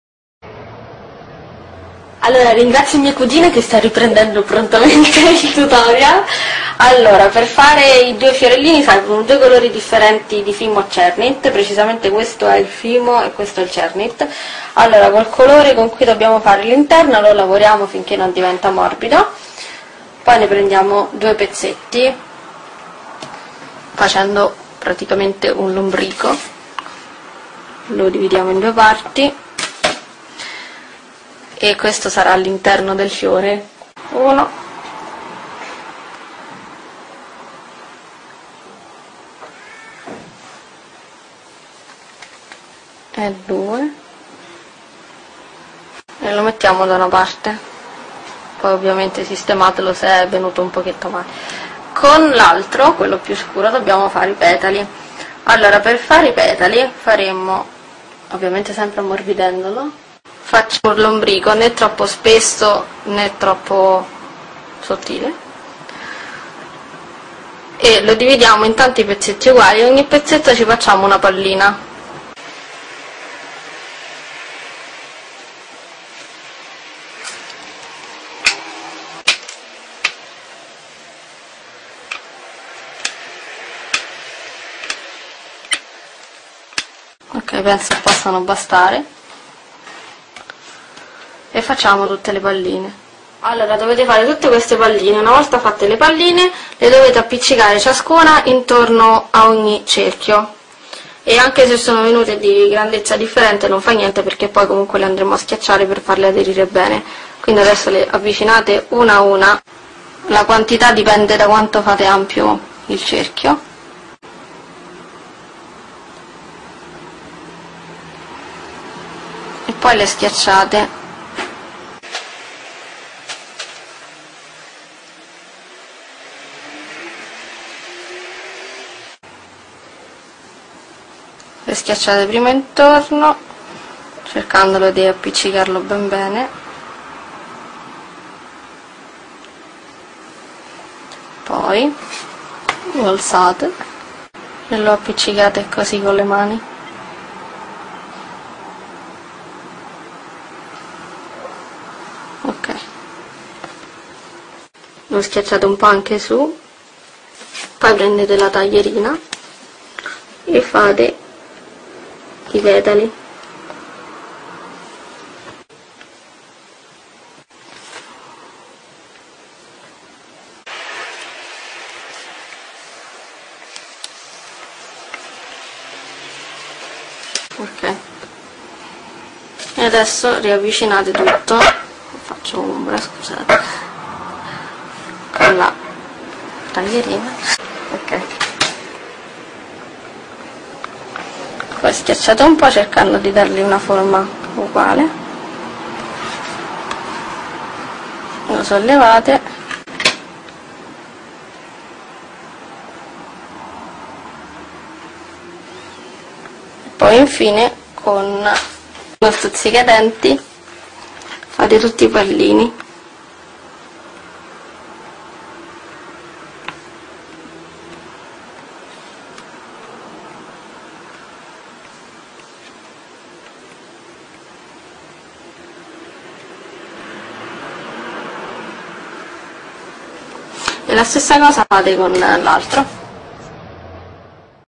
Tutorial 6 • fiore margherita in cernit e fimo - flower polymer clay
tutorial: come creare un fiore in fimo e cernit how to create a polymer clay flower